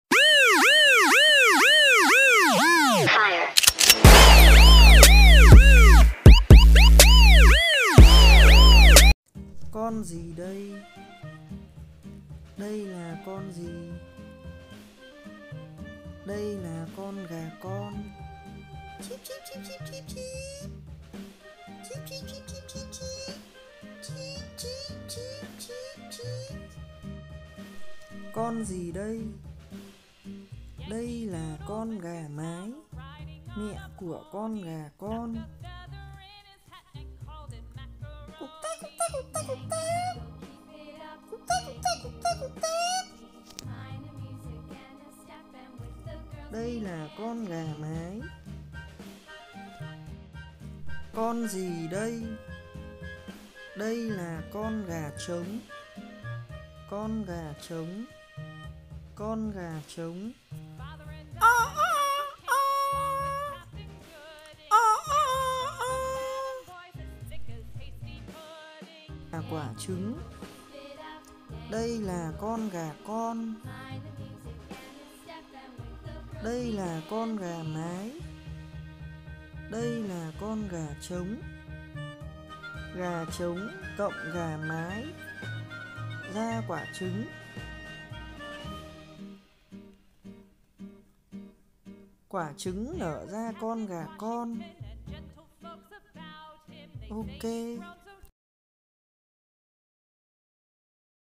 Con Gà - Học Các Con Vật Bằng Hình Ảnh Cho Trẻ Em | Gia Đình Nhà Gà - Langmoontv
Con Gà - Các Con Vật Cho Trẻ Em  | Gia Đình Nhà Gà - Langmoontv
 dậy trẻ em về các con vật
Con gà mái,con gà trống,con gà con ,quả trứng ,